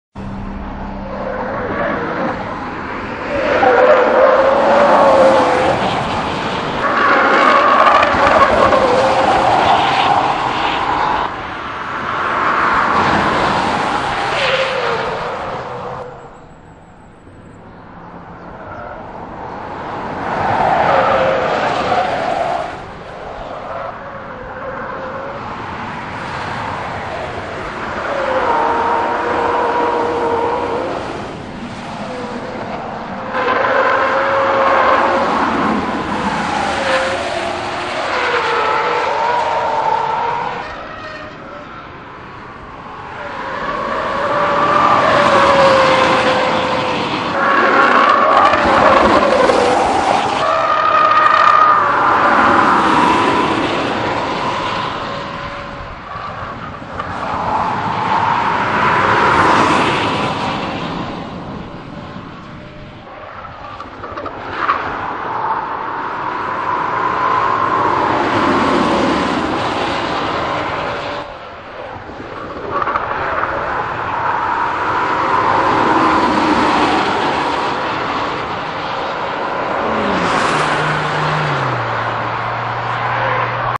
2014 BMW X5 spied in action
BMW engineers have been busy testing a prototype for the next generation X5 SUV on the famed Nurburgring in Germany. Luckily for us, our spy photographers were on hand to catch it in this all-access video.